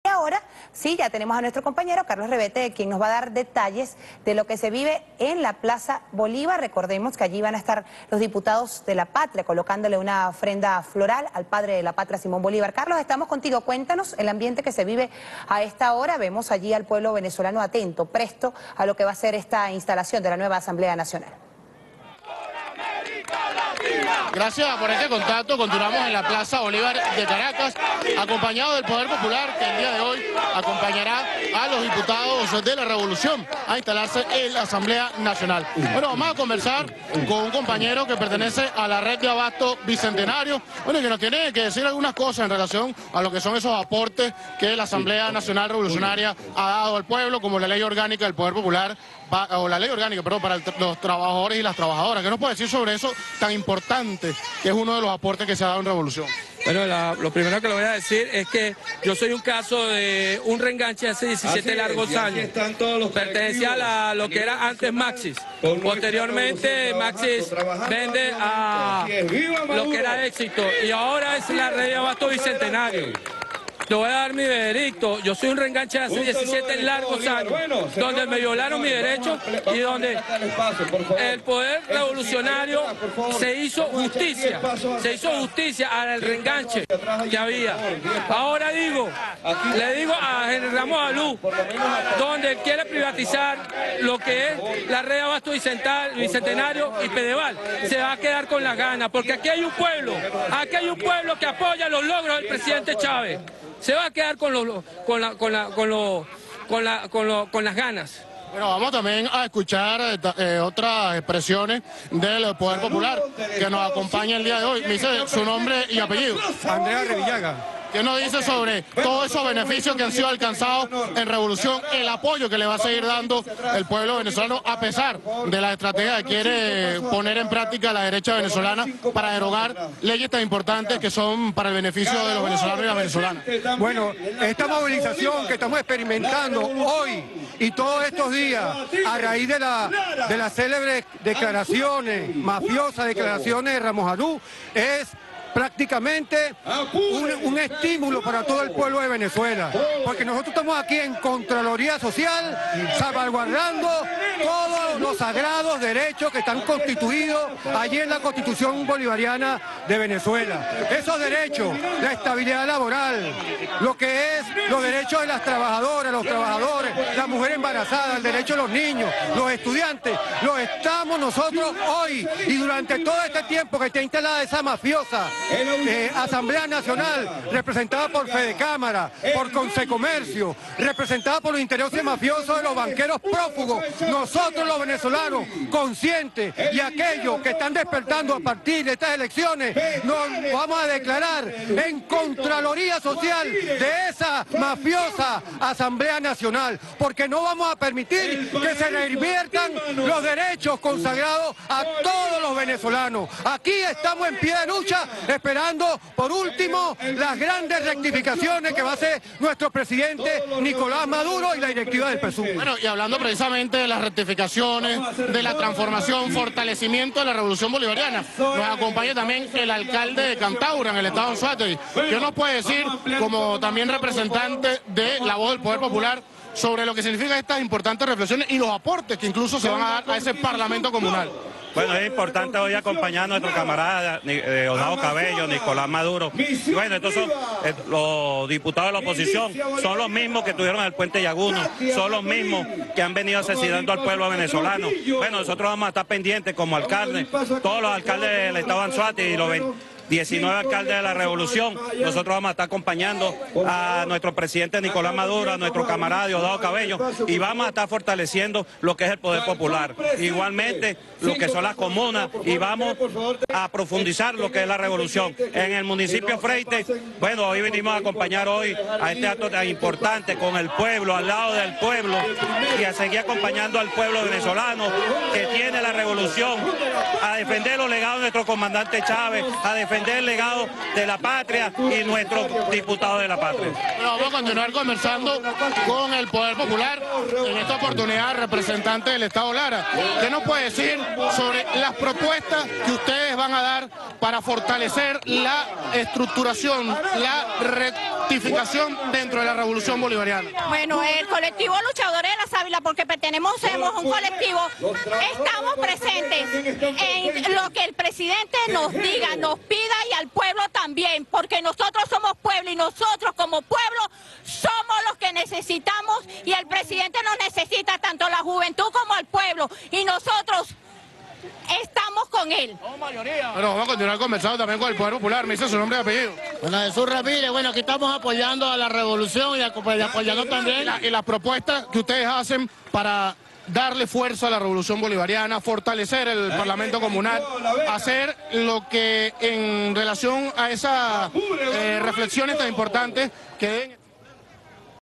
En Plaza Bolìvar se manifiestan ante juramentación de la derecha en el Parlamento, 5 enero 2016
Transmisiòn de VTV